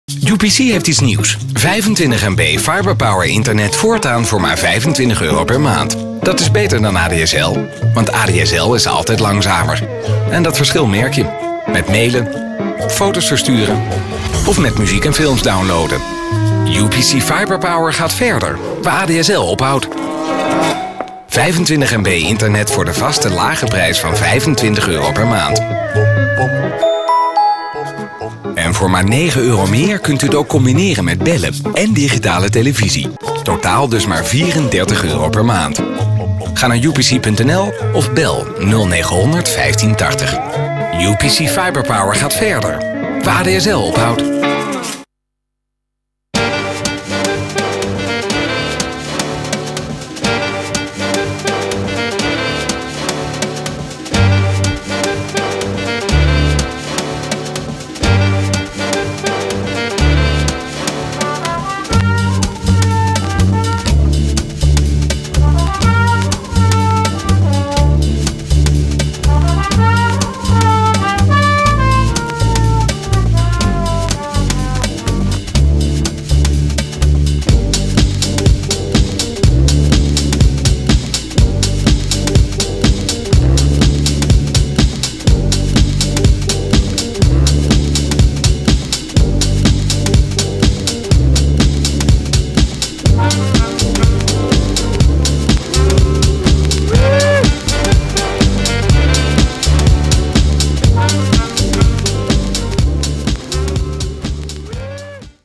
UPC Fiber Power Game i-Ad
This red-button interactive advertisment allows viewers to enter a microsite and play a game in response to a commercial for UPC's fast internet packages. In the game, the viewer races against ADSL and UPC Fiber Power 120 to see which is fastest.